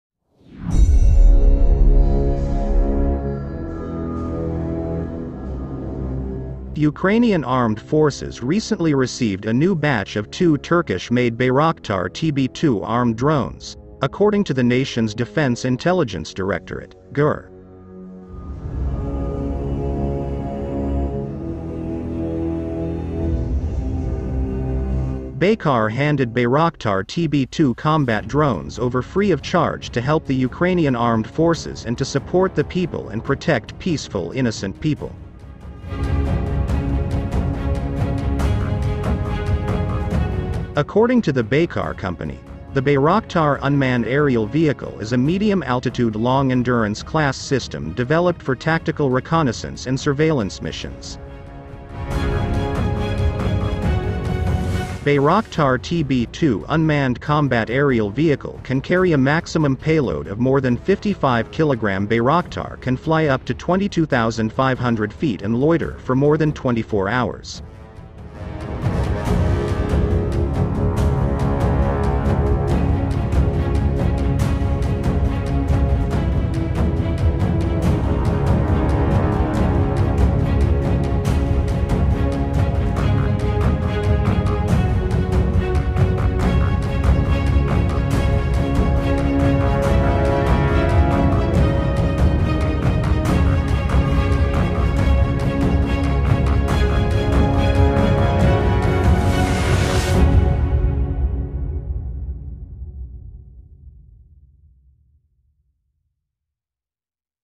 Russia Shocked; The Ukrainian military received a new batch of Bayraktar TB2 drones from Baykar.
The Ukrainian military received a new batch of Bayraktar TB2 drones for free from Baykar.